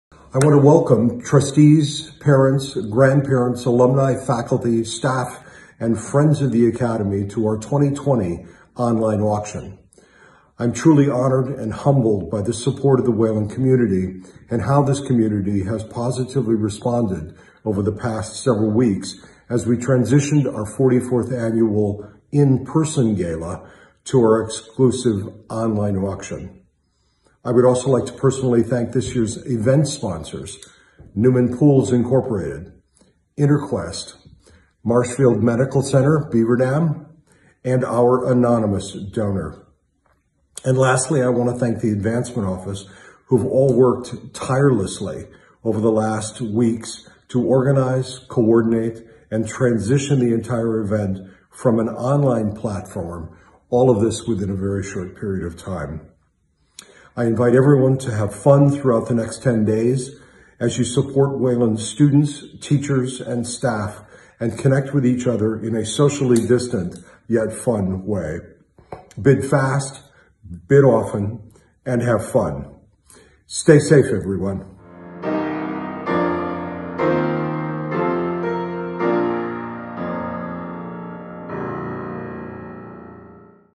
Welcome to the Wayland Online Auction
Wayland Academy's largest fundraiser of the year has become an online event due to the COVID-19 pandemic. While we'll miss greeting everyone personally, Head of School Joseph Lennertz H`86 welcomes everyone to the online auction this year.